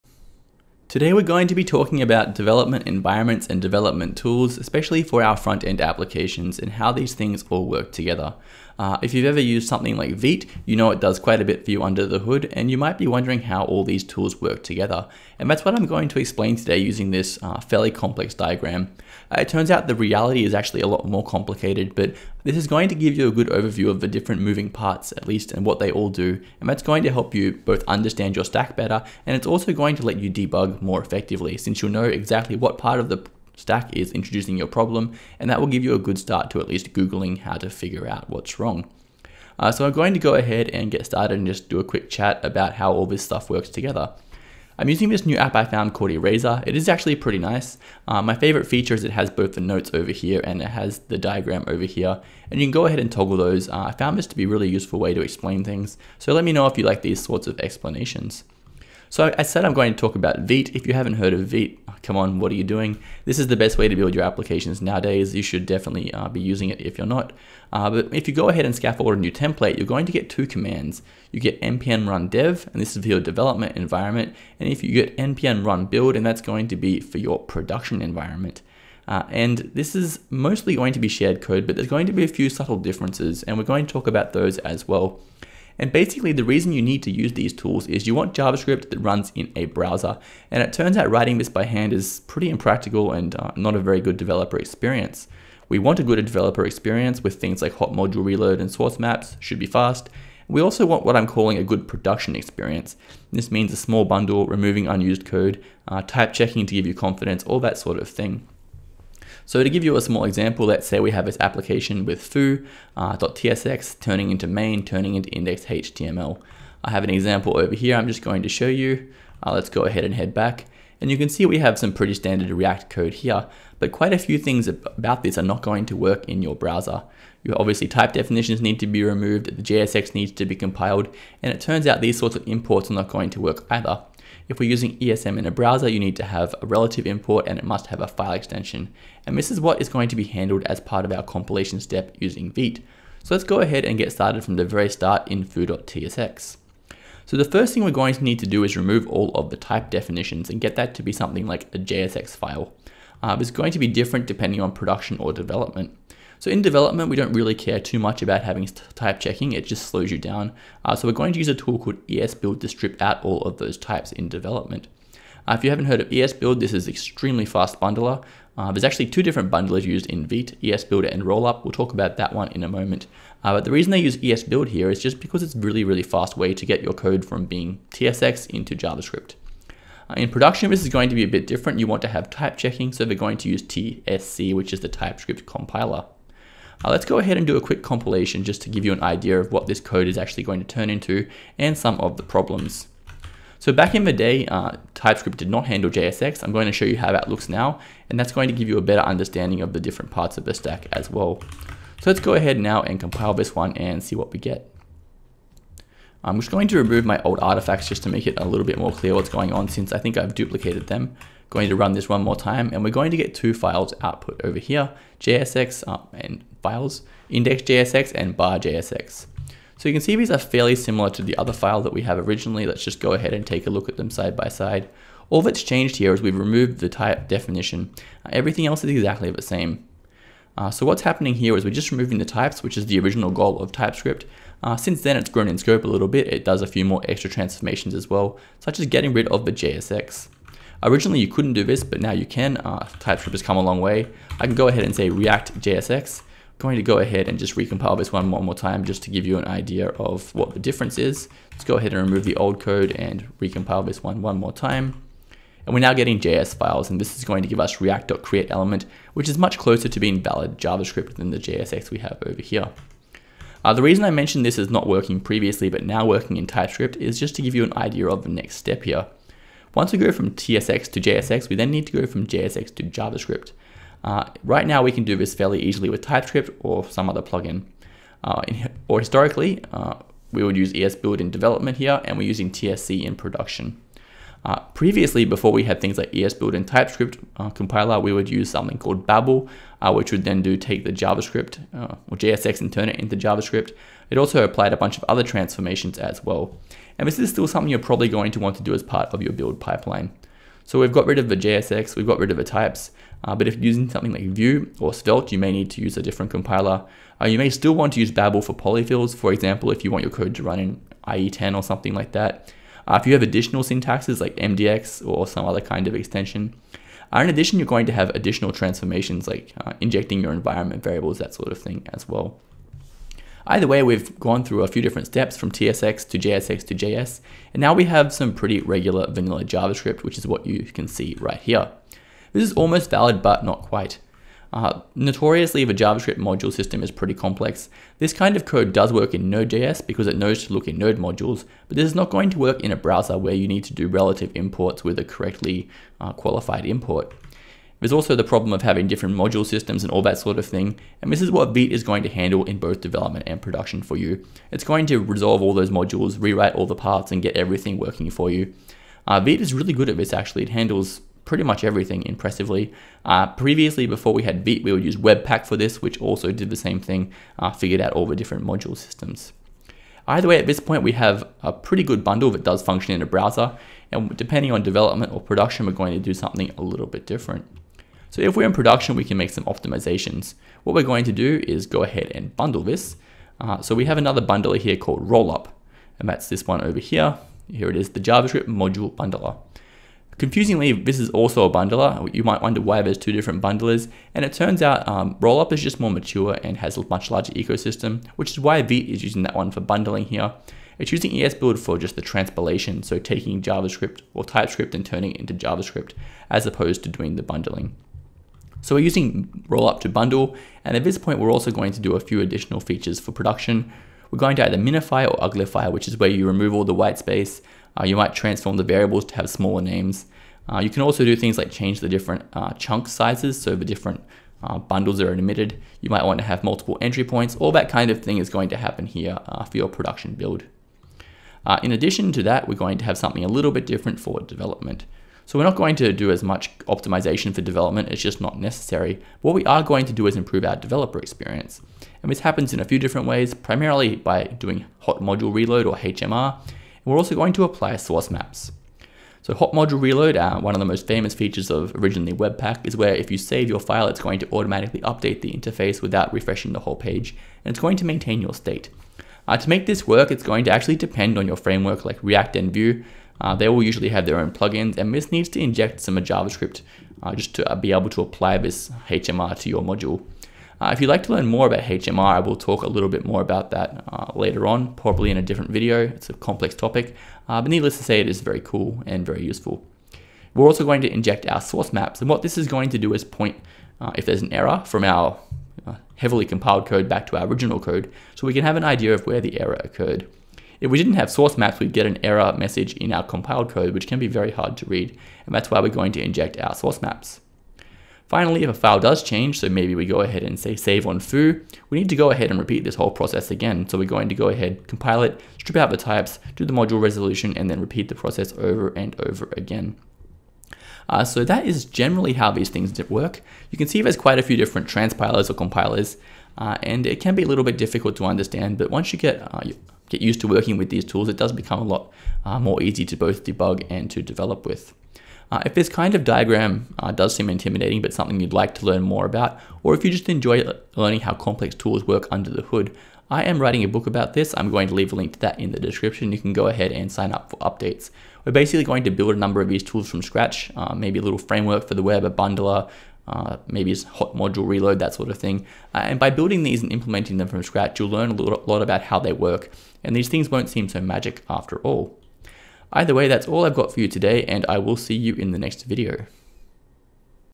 Visual Guide to the Modern Frontend Toolchain (Vite)
🎉 NEW book on tooling (build your own tools!) Coming 2024, register for updates)

I talk about Vite, the best toolchain for building frontend apps right now, which combines a number of other tools under the hood with with some extra goodies for a great development experience.